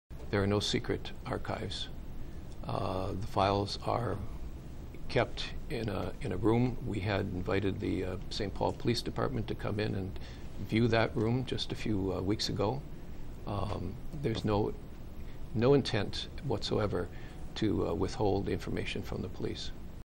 Deposition of Archbishop John Nienstedt (excerpt 5)
"There are no secret archives."

Archbishop John Nienstedt answered questions about his handling of clergy sexual abuse in the Archdiocese of St. Paul and Minneapolis during an April 2, 2014, deposition.

The questioning comes as part of a lawsuit brought by a man who says he was sexually abused by the Rev. Thomas Adamson in the 1970s.